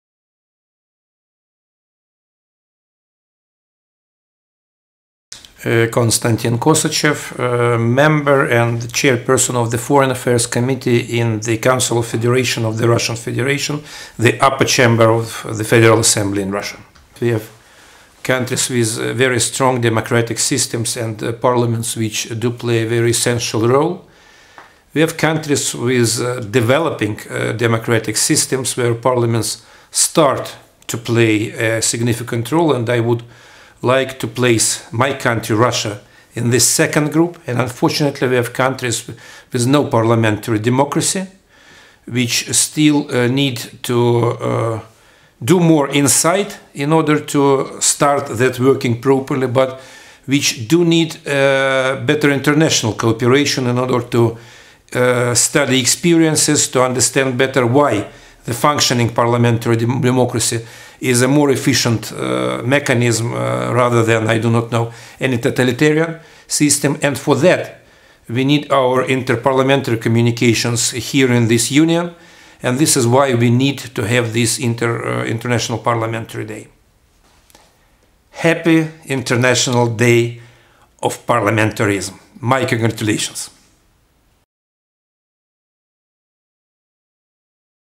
International Day of Parliamentarism: Konstantin Kosachev, VP, IPU Executive Committee & Russian MP
Konstantin Kosachev on the different kinds of parliamentary democracy on the occasion of the first International Day of Parliamentarism, 30 June 2018.